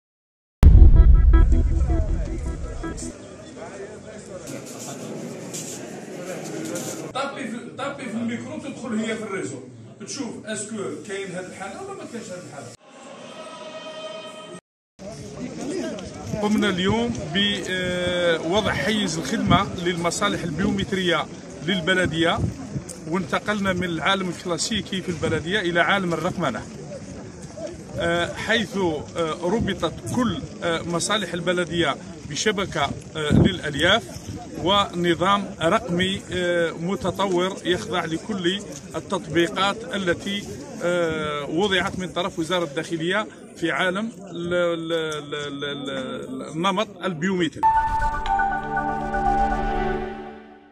إليزي : وضع حيز الخدمة مصلحة جديدة للحالة المدنية والوثائق البيومترية
إليزي (وأج) – دخلت مصلحة جديدة للحالة المدنية  والوثائق البيومترية  حيز الخدمة، من خلال تدشين مرفق جديد من شأنه  تحسين الخدمة العمومية للمواطنين.